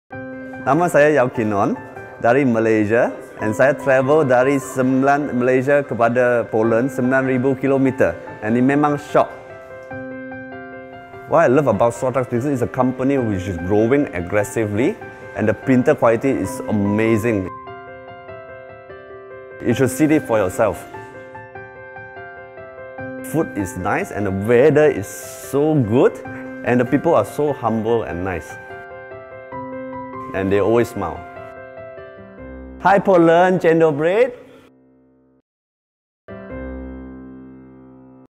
Experience Zortrax with Trinity 3D Solutions, Malaysia
What Malaysians love about Poland and Zortrax? Find out in the second episode of ExperienceZortrax series. This time we’re meeting with Trinity 3D Solutions, our Official Reseller in Malaysia 🇲🇾.
Terima kasih!